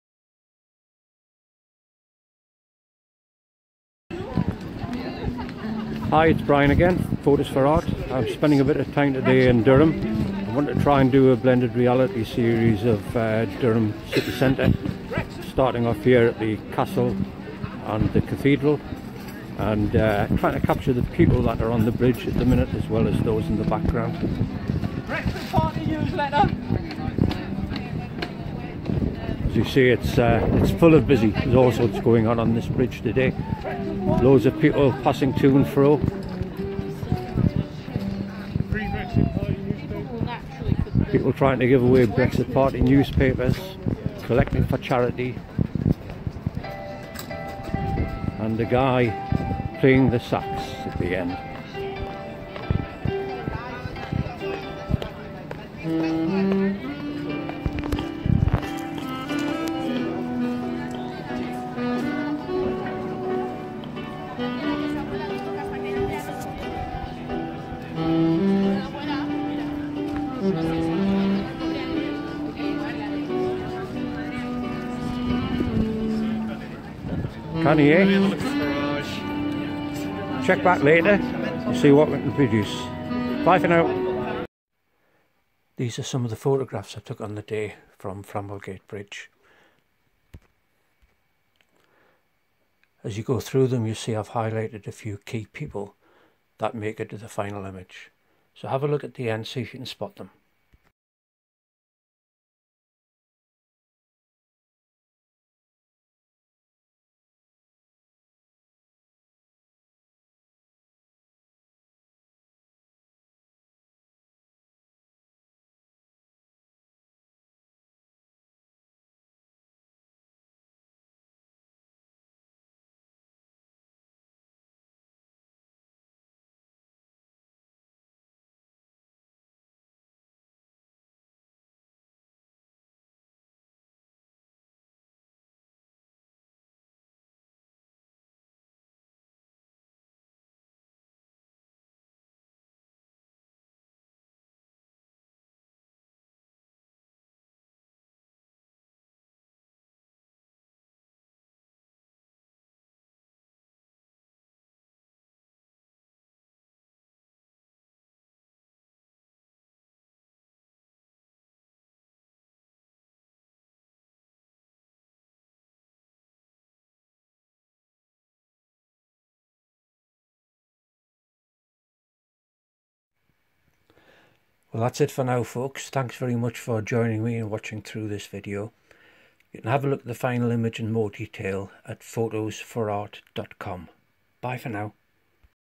Blended Reality shoot of Durham City's Castle & Cathedral From Framwellgate Bridge
Durham's Castle and Cathedral
I showcase my photo-shoot for a limited-edition Fine Art print of Durham's castle and cathedral from Framwellgate Bridge. I take you through the main scene, a selection of key images, highlight some of the main characters that make the final cut and finally show you the completed image.